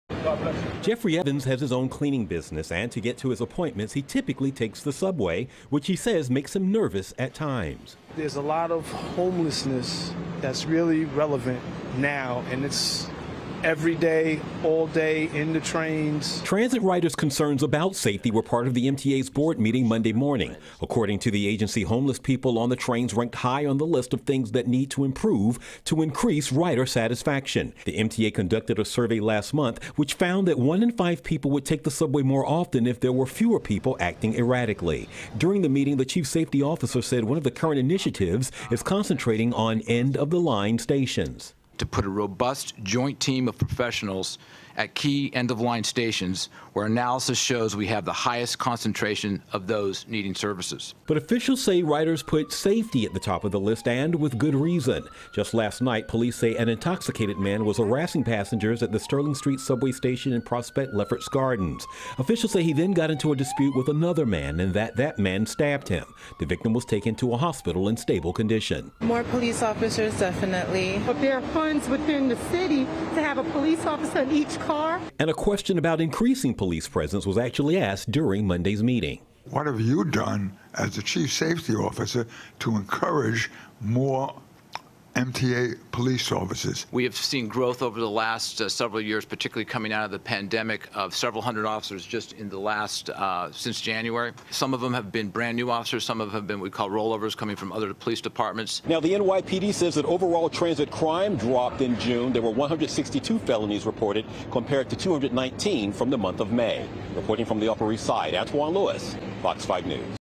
NYC subway safety and homelessness
The MTA Board met on Monday to discuss subway safety and outreach services. The MTA says "homeless people on the trains" ranked high on a list of things that need to improve to increase rider satisfaction.